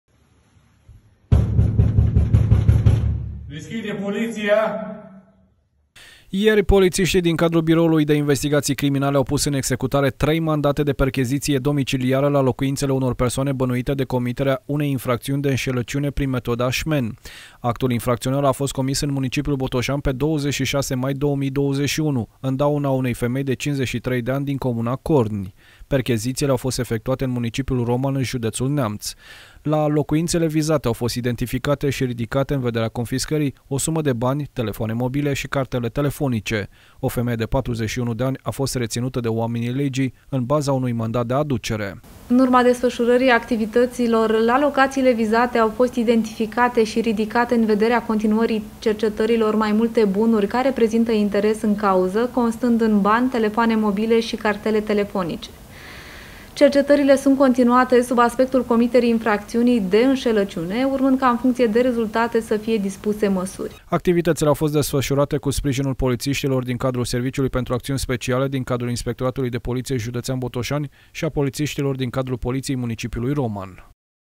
PACALITA PRIN METODA SMEN
O femeie din Corni a fost păcălită prin metoda “şmen” de mai mulţi indivizi din municipiul Roman. Astfel de înşelăciuni se bazează pe inducerea în eroare a victimei. Escrocii solicită schimbarea mai multor bancnote, însă ajung să se răzgândească, moment în care sustrag o sumă mai mare de bani de la victimă. Ieri, la persoanele suspecte de comiterea acestor fapte au fost efectuate percheziţii domiciliare. O femeie din Roman a fost reţinută de poliţişti.